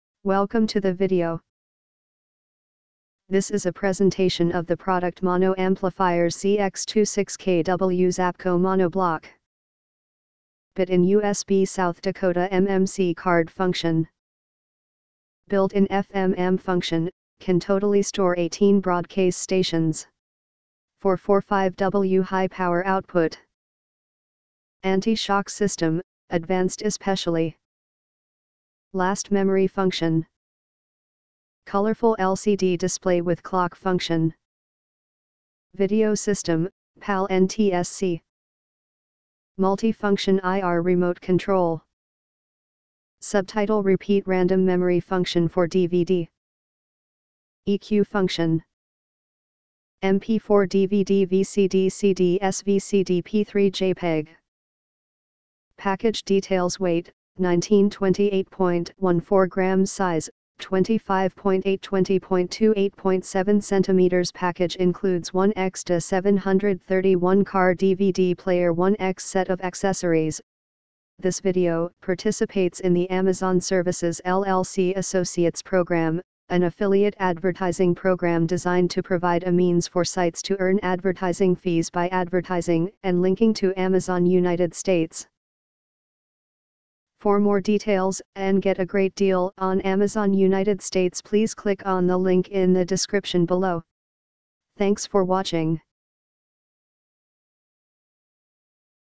HFK7318 DA731 PALNTSC
Pгоduсt Dеsсгіptіоn
* Buit-in USB/SD/MMC card function. * Built-in FM/AM function, can totally store 18 broadcase stations. * 4*45W high power output. * Anti-shock system, advanced ESP (DVD: 5s, CD: 15s, MP3: 60s). * Last memory function (LMF). * Colorful LCD display with clock function. * Video system: PAL/NTSC. * Multi-function IR remote control. * Subtitle/repeat/random/memory function for DVD. * EQ function. * MP4/DVD/VCD/CD/SVCD/P3/JPEG. Package Details Weight: 1928.14 g Size: 25.8*20.2*8.7 cm Package Includes 1 x DA-731 Car DVD Player 1 x Set of Accessories

* Buit-in USB/SD/MMC card function
Free Worldwide Shipping with registred airmail service - Livraison gratuite
Thank you for visiting eTreasures